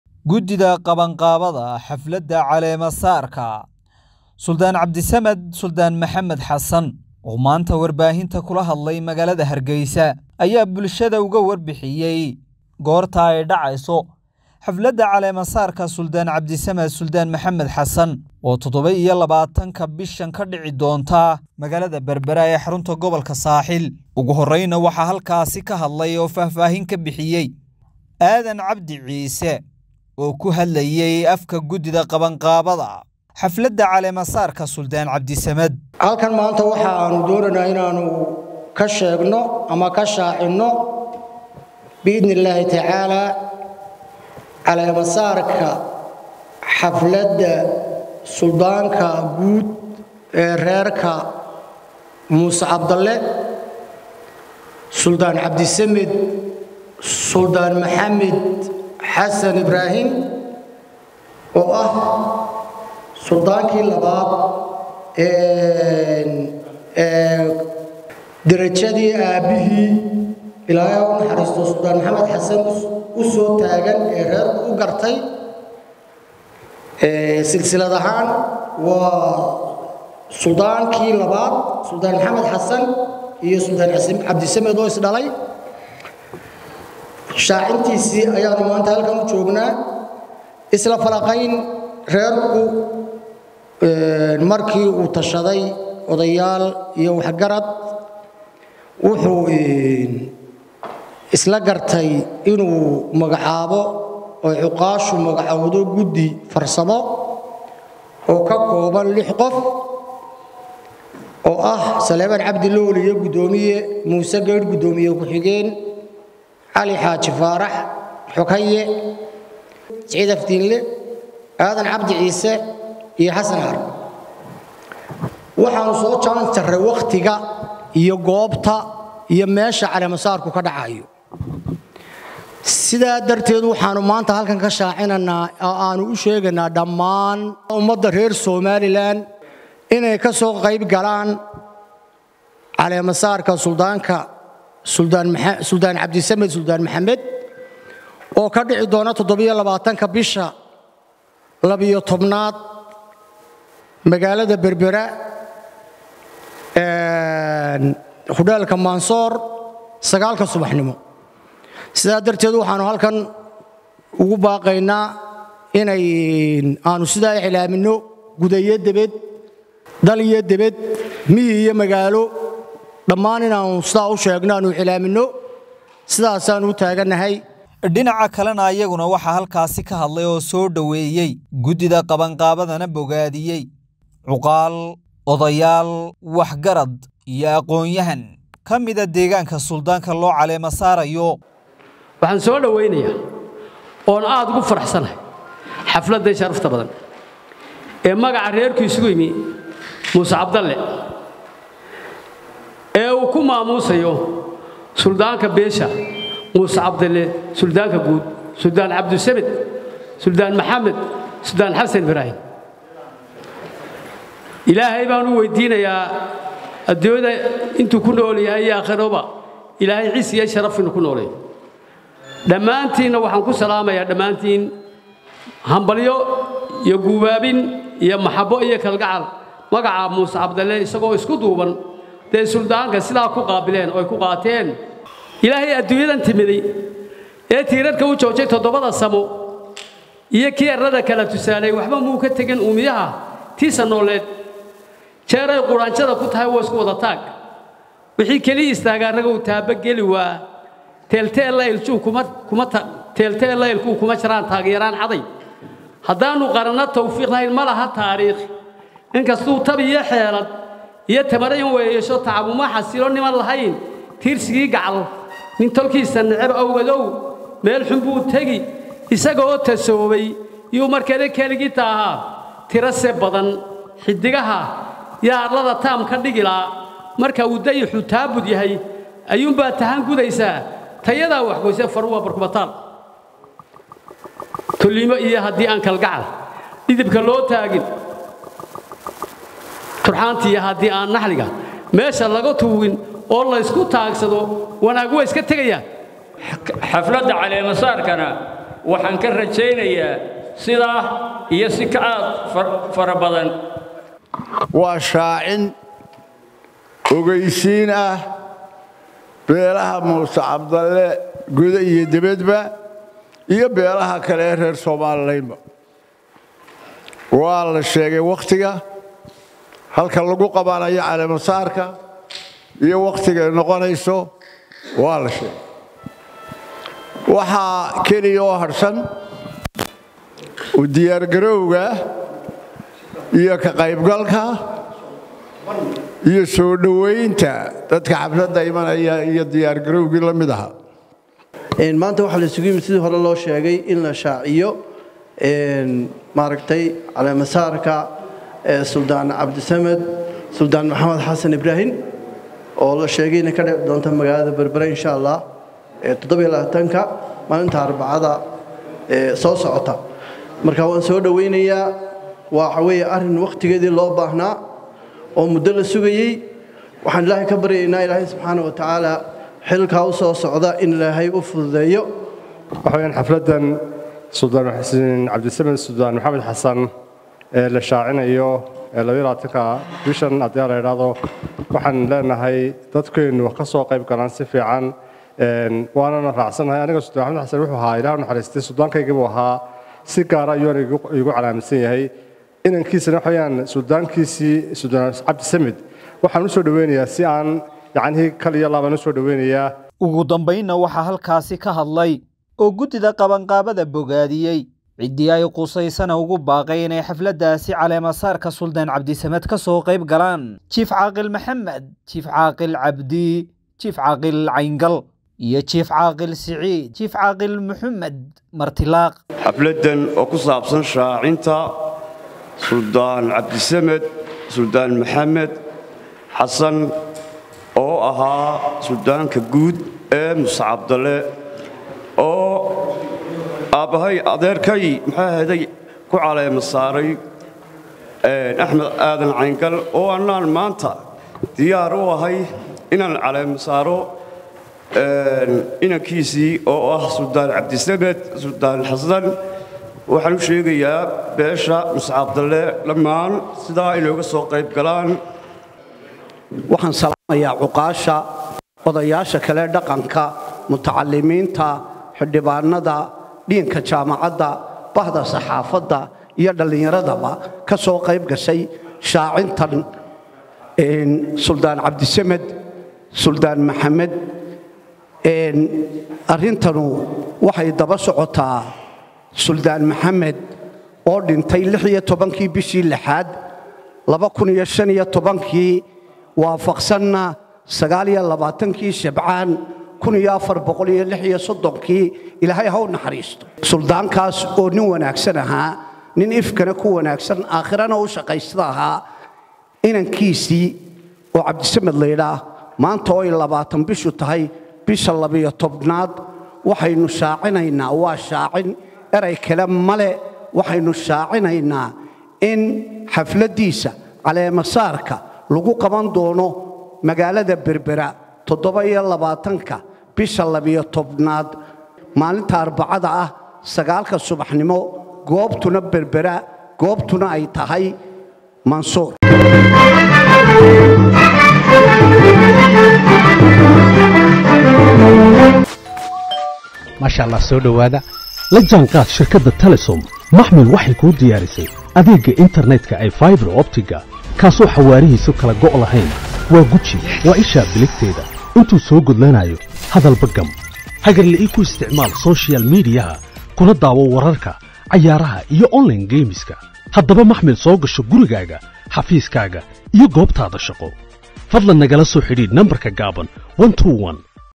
xafladda Caleemo-saarka Suldaan Cabdisamed Suldaan maxamed Xassan ayaa dhacaysa 27\12\2023 Berbera
Gudidda qaban qaabada xafladda caleemo saarka Suldaan Cabdisamed Suldaan Maxamed Xasan ayaa sheegay wakhtiga la qabanayo xafladda